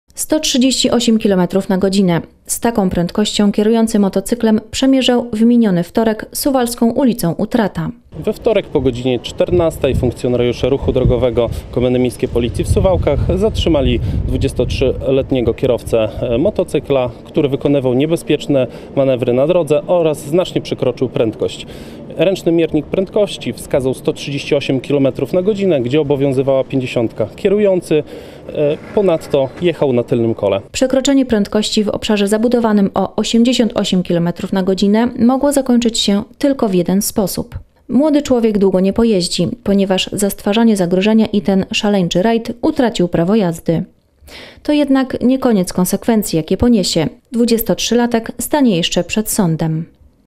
Kaskader już nie pojeździ |TV SUWAŁKI
138 km na godzinę – z taką prędkością kierujący motocyklem przemierzał w miniony wtorek suwalską ul. Utrata.